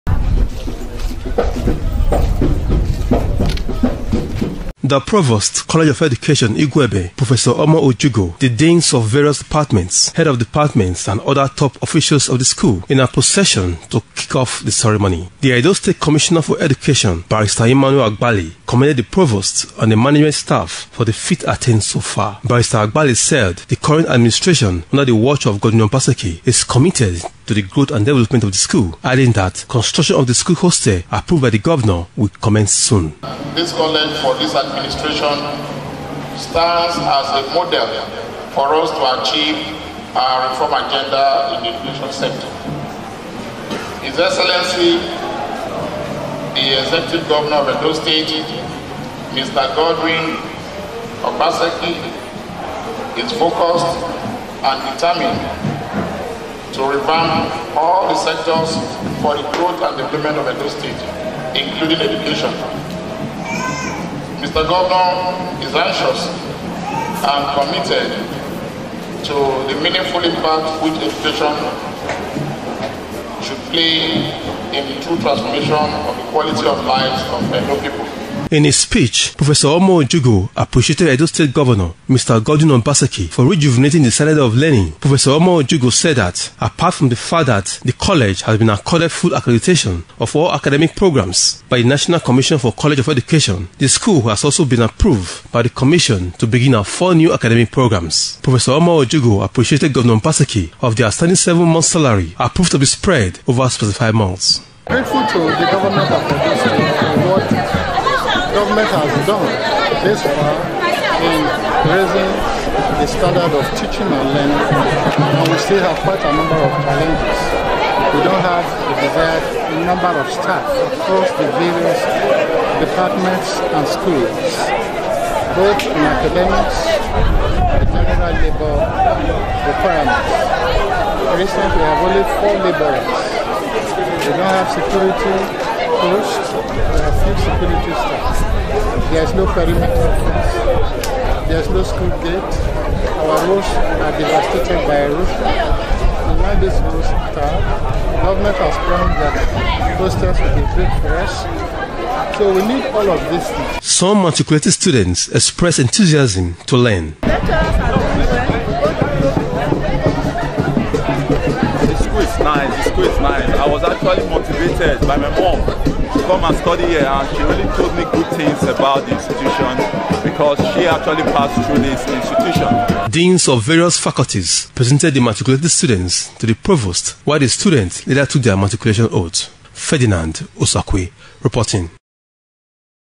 College of Education, Igueben holds 13th Matriculation Ceremony
College of Education Igueben, Edo State has held its thirteenth matriculation ceremony to admit students into NCE programme for 2018/2019 academic session.

Edo Central Bureau Chief, Fedinard Osakue reports that the provost of the institution, Professor Omo Ojugo used the opportunity to read out achievements the college has attained so far.